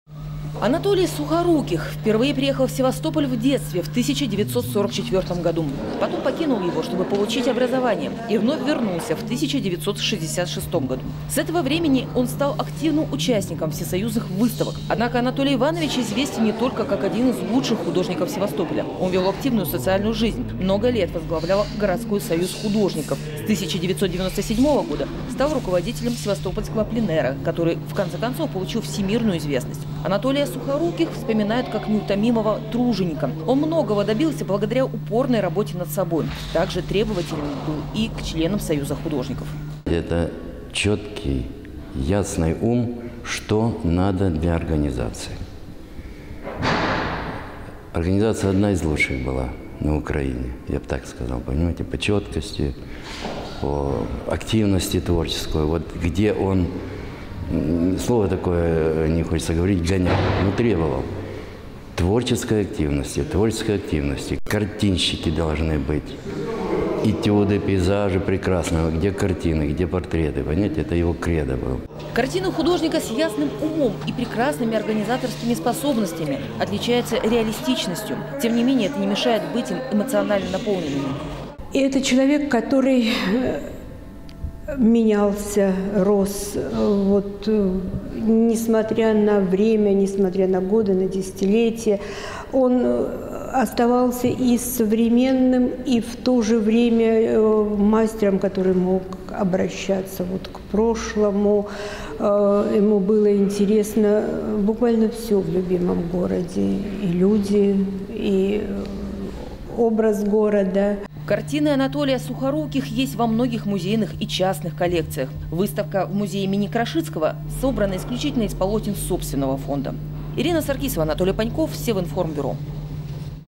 19.12.2015 Художественная выставка памяти Анатолия Сухоруких
В Севастопольском художественном музее имени Михаила Павловича Крошицкого открылась выставка картин. На ней представлены 21 полотно Анатолия Ивановича Сухоруких. Этот севастопольский художник, который долгие годы жил и работал в городе, ушел из жизни 21 августа этого года. 20 декабря ему исполнилось бы 80 лет.